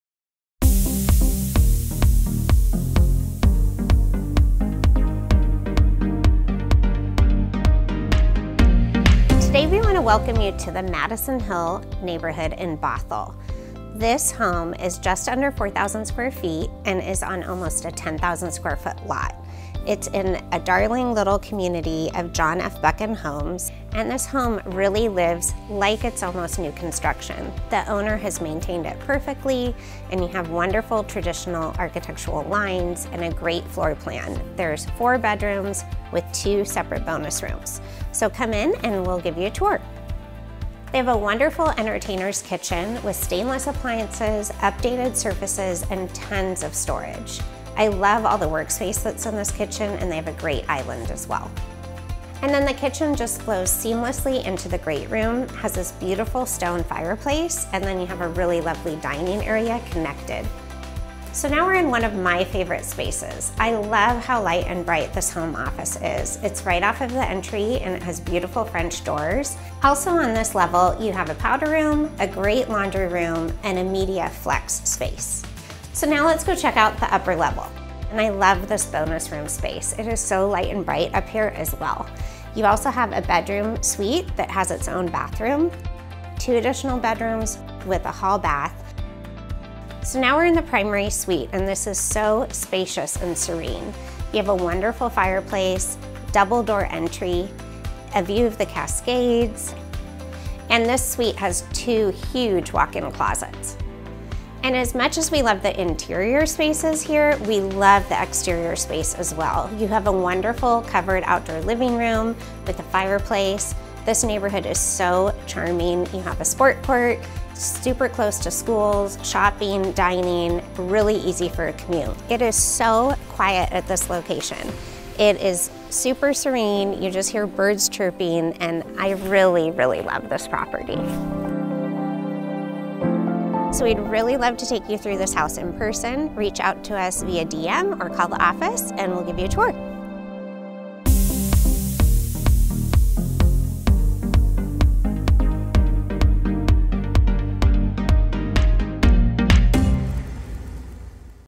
Vlog 076: Join Moya in Bothell’s Madison Hill Neighborhood
On TFVlog Episode 076, join Moya in the cherished Madison Hill neighborhood and discover a wonderful blend of distinctive spaces and Bothell tranquility at this John F. Buchan resale. A gracious entry sets the stage for timeless elegance with light-filled open spaces designed for both formal and informal gatherings. Nestled on a spacious lot and boasting some lake and mountain vistas, the floor plan is ideal for the way we live today with a chef’s kitchen (including double ovens + updated tile & backsplash) that opens to the great room with roaring stone fireplace as well as the covered outdoor living space—ready for barbecue season! 4 bedrooms plus office & two bonus rooms; flexible spaces for those working from home. A serene primary suite is served by two walk-in closets and a 5-piece bath. The expansive, fully fenced backyard is ready for gathering, gardening, and play (you’ll also love the neighborhood playground & sport court!) Convenient commute to Redmond, Bellevue, Seattle, and Everett. Award-winning Northshore Schools. This is the one you’ve been waiting for...